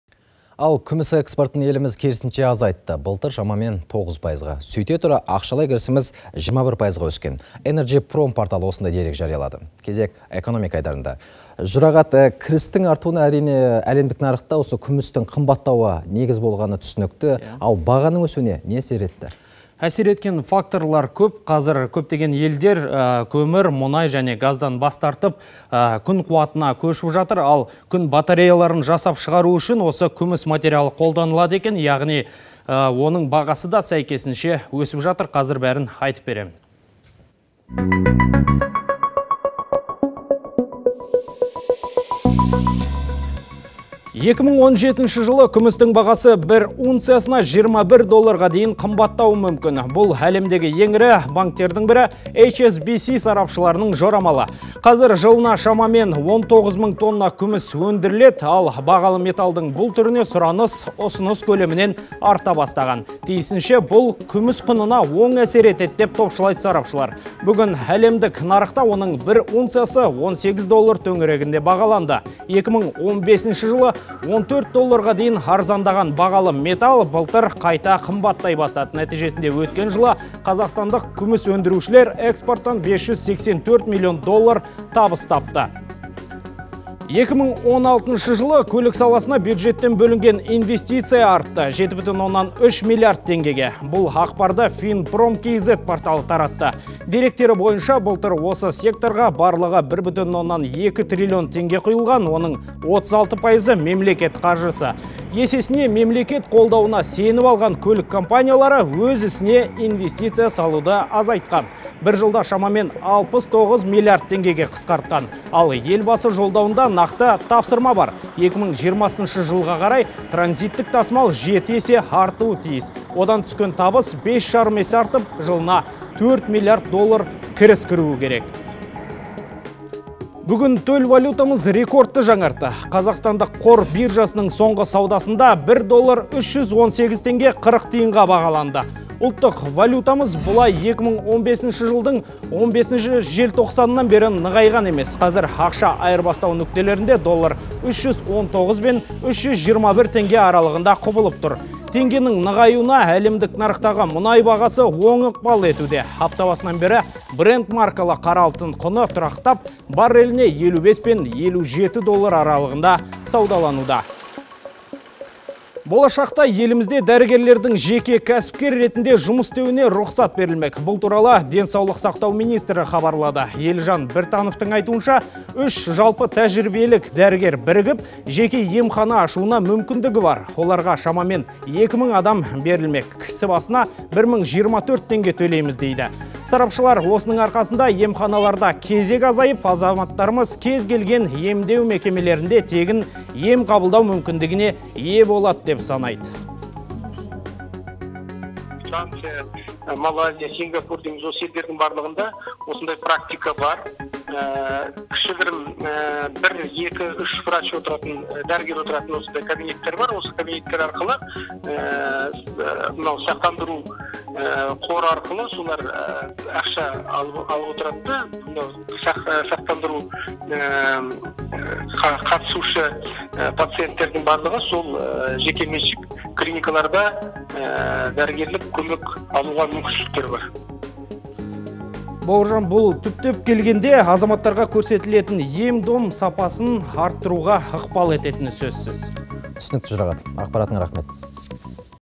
Елімізде күміс экспорты 9 пайызға азайған
Жүргізуші:

-Сөйте тұра ақшалай кірісіміз 21 пайызға өскен. «Энерджипром» порталы осындай дерек жариялады. Кезек экономика айдарында. Жұрағат, кірістің артуына әлемдік нарықта күмістің қымбаттауы әсер еткені түсінікті. Ал бағаның өсуіне не себеп?

Жұрағат Баман, тілші:

-Әсер еткен факторлар көп. Солардың бірі қазір көптеген ел мұнай, көмір және газдан бас тартып, күн энергиясын пайдалануға ден қойып отыр. Ал күн батареяларын жасауда күміс қолданылады. Демек, бағалы металдың бұл түріне сұраныс артпаса, кеми қоймайды.

2017 жылы күмістің бағасы бір унциясына 21 долларға дейін қымбаттауы мүмкін. Бұл – әлемдегі ең ірі банктердің бірі «Эйч-Эс-Би-Си» сарапшыларының жорамалы. Қазір жылына шамамен 19 мың тонна күміс өндіріледі. Ал бағалы металдың бұл түріне сұраныс ұсыныс көлемінен арта бастаған. «Тиісінше бұл күміс құнына оң әсер етеді деп топшылайды», – сарапшылар. Бүгін әлемдік нарықта оның бір унциясы 18 доллар төңірегінде бағаланды. 2015 жылы 14 долларға дейін арзандаған бағалы металл былтыр қайта қымбаттай бастады. Нәтижесінде өткен жылы қазақстандық күміс өндірушілер экспорттан 584 млн доллар табыс тапты.

2016 жылы көлік саласына бюджеттен бөлінген инвестиция 7,3 млрд теңгеге артты. Бұл ақпарды finprom.kz порталы таратты.  Деректері бойынша, былтыр осы секторға барлығы 1,2 трлн теңге құйылған. Оның 36 пайызы – мемлекет қаржысы. Есесіне мемлекет қолдауына сеніп алған көлік компаниялары өз ісіне инвестиция салуды азайтқан. Бір жылда шамамен 69 млрд теңгеге қысқартқан. Ал Елбасы Жолдауында нақты тапсырма бар. 2020 жылға қарай транзиттік тасымал 7 есе артуы тиіс. Одан түскен табыс 5,5 есе артып, жылына 4 млрд доллар кіріс кіруі керек.

Бүгін төл валютамыз рекордты жаңартты. Қазақстандық қор биржасының соңғы саудасында 1 доллар 318 теңге 40 тиынға бағаланды. Ұлттық валютамыз бұлай 2015 жылдың 15-ші желтоқсанынан бері нығайған емес. Қазір ақша айырбастау нүктелерінде доллар 319  бен 321 теңге аралығында құбылып тұр. Теңгенің нығаюына әлемдік нарықтағы мұнай бағасы оң ықпал етуде. Апта басынан бері Брент маркалы қара алтын құны тұрақтап, барреліне 55 пен 57 доллар аралығында саудалануда.

Болашақта елімізде дәрігерлердің жеке кәсіпкер ретінде жұмыс істеуіне рұқсат берілмек. Бұл туралы Денсаулық сақтау министрі хабарлады. Елжан Біртановтың айтуынша, 3 жалпы тәжірибелік дәрігер бірігіп, жеке емхана ашуына мүмкіндігі бар. Оларға шамамен 2 мың адам берілмек. Кісі басына 1024 теңге төленеді. Сарапшылар: «Осының арқасында емханаларда кезек азайып, азаматтарымыз кез келген емдеу мекемелерінде тегін ем қабылдау мүмкіндігіне ие болады», –  деп санайды.